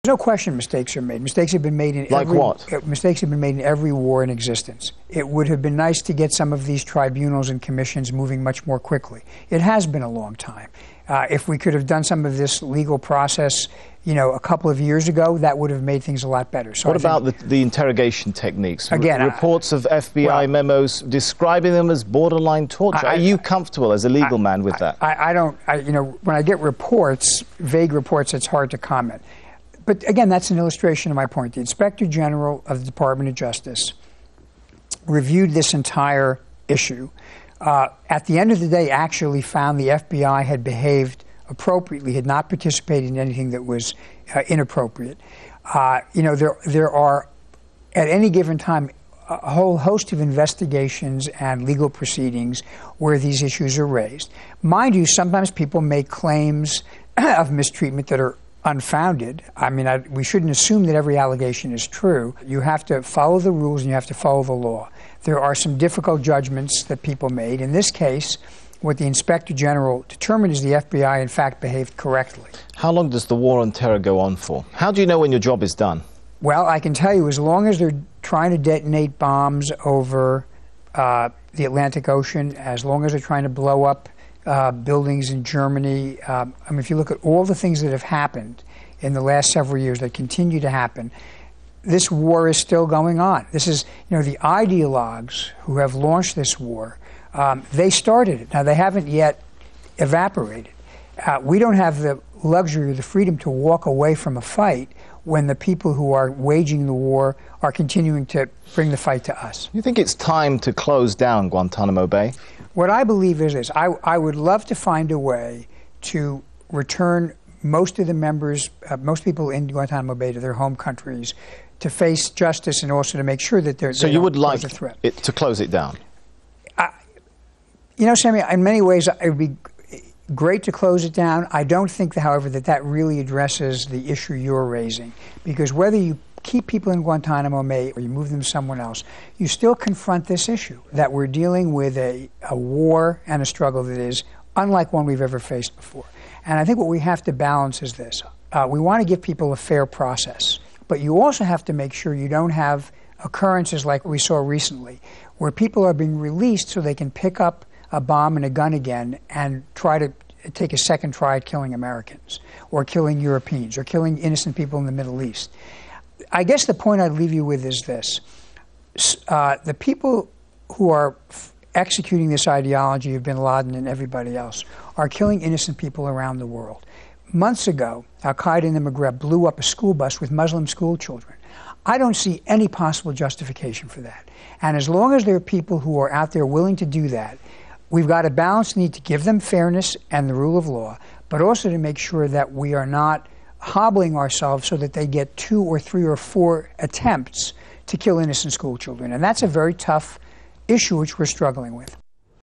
US homeland security chief talks to Al Jazeera - 27 May 2008
Human rights groups say Guantanamo Bay is a stain on the reputation of the United States.

There are still 200 people being held there, without charge or trial.

President Bush has hinted he now wants it closed - so has his Homeland Security chief.

Al Jazeera's Sami Zeidan spoke to Michael Chertoff and asked him whether the US had made mistakes in its treatment of prisoners.